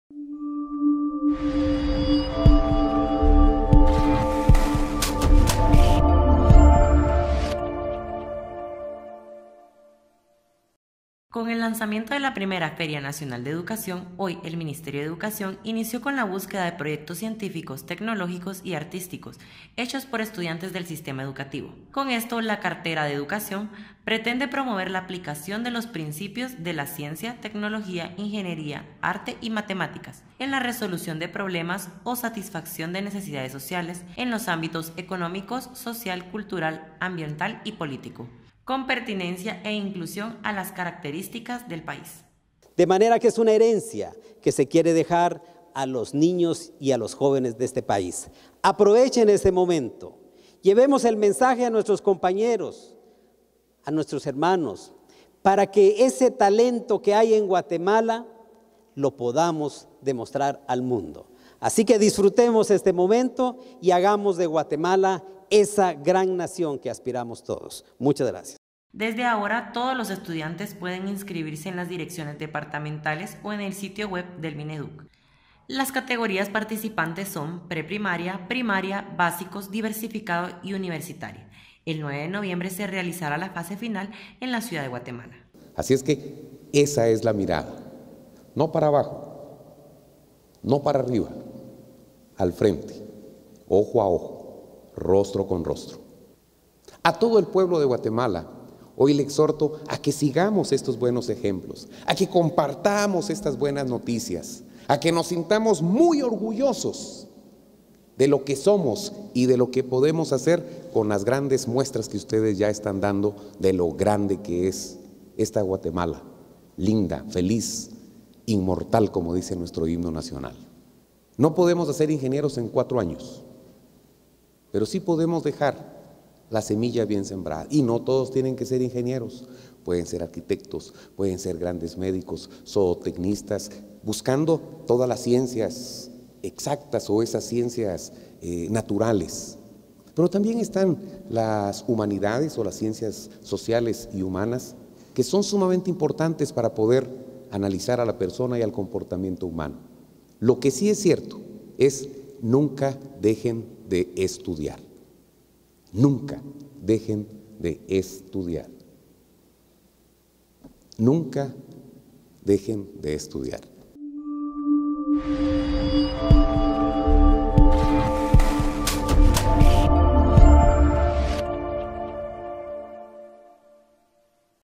Mineduc inicia búsqueda de proyectos de innovación
Con el lanzamiento de la Primera Feria Nacional de Educación, hoy el Ministerio de Educación inició con la búsqueda de proyectos científicos, tecnológicos y artísticos hechos por estudiantes del sistema educativo.

Con esto, la cartera de educación pretende promover la aplicación de los principios de la ciencia, tecnología, ingeniería, arte y matemática en la resolución de problemas o satisfacción de necesidades sociales en los ámbitos económico, social, cultural, ambiental y político, con pertinencia e inclusión a las características del país.

Desde ahora, todo los estudiantes pueden inscribirse en las direcciones departamentales o en el sitio web del Mineduc.

Las categorías participantes son: preprimaria, primaria, básicos, diversificado y universitaria. El 9 de noviembre se realizará la fase final en la ciudad de Guatemala.